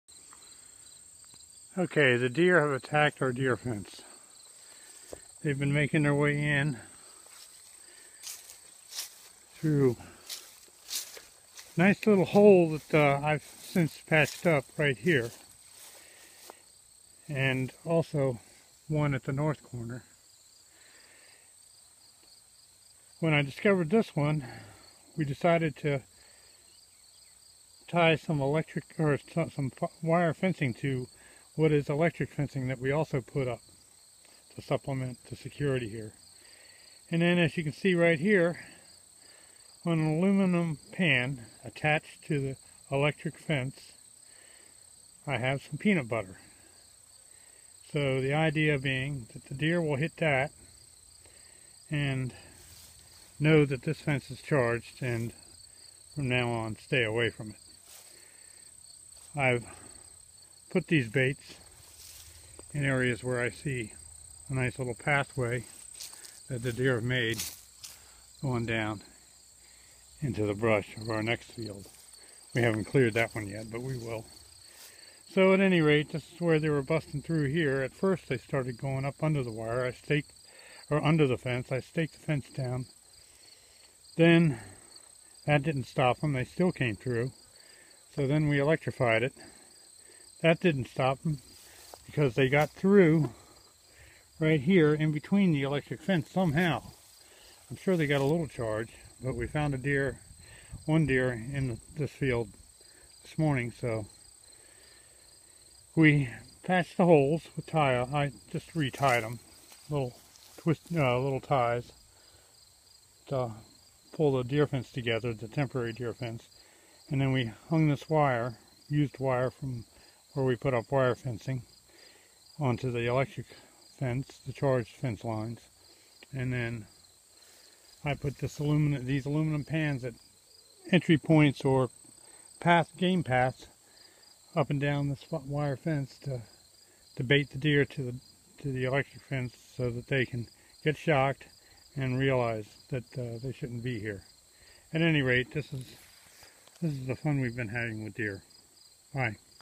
How to teach deer not to hit a charged fence wire
In order to train the deer off a fence we 1st electrify it then bait it.
Hopefully this works